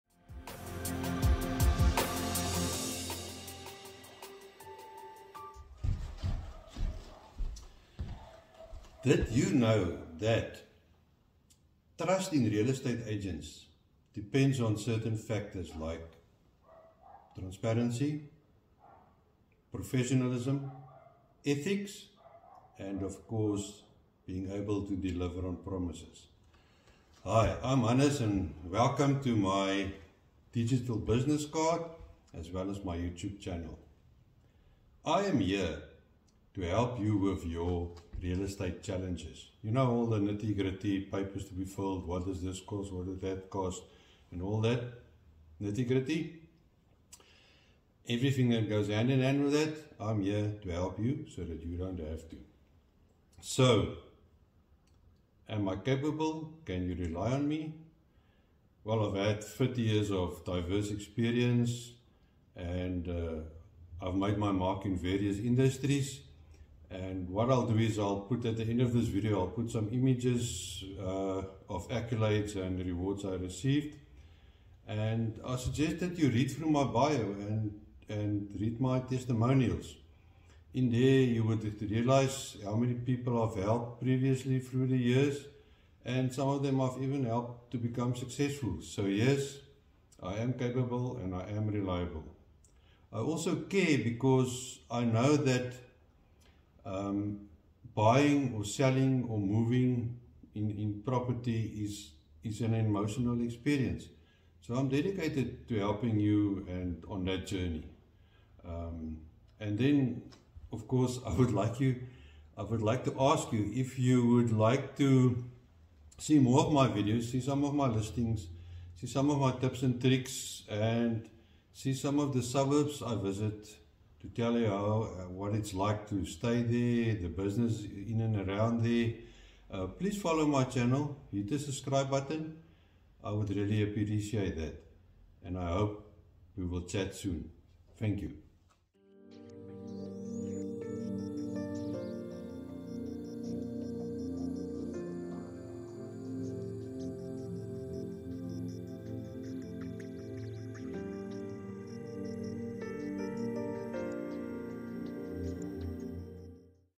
Unlocking Real Estate Success with Hannes: Your Trusted Guide
Need a capable, able, caring and trusted agent?

"Welcome to Hannes' Real Estate World! 🏡

Meet Hannes, your trusted guide in the exciting realm of real estate. In this introduction video, Hannes invites you to embark on an exceptional real estate journey. With over 30 years of diverse experience, Hannes brings a wealth of expertise to the table. His rich tapestry of accomplishments spans various industries, including being named Best Franchiser in 1997 and 1999.

Hannes blends his seasoned professionalism with a touch of easygoing charm, making your real estate experience not only efficient but enjoyable. He's dedicated to delivering top-notch results while keeping things down-to-earth.

But Hannes isn't just a real estate expert; he's also a devoted family man. He finds immense joy in spending time with his wife and cherishing moments with his five grandchildren. Family is at the core of his values, driving his commitment to building a better future for generations to come.

In this video, you'll discover Hannes' passion for real estate, his commitment to clients, and his vision for unlocking endless possibilities in the world of real estate. Whether you're a first-time buyer, seasoned investor, or a seller aiming to maximize property value, Hannes is your trusted partner.

Join Hannes and eXp to step into a world of seamless and rewarding real estate adventures.

Are you ready to make your real estate journey a breeze? Connect with Hannes today and explore the world of real estate with confidence.

📞 Hannes Gerber
eXp South Africa. Licence No. 1253240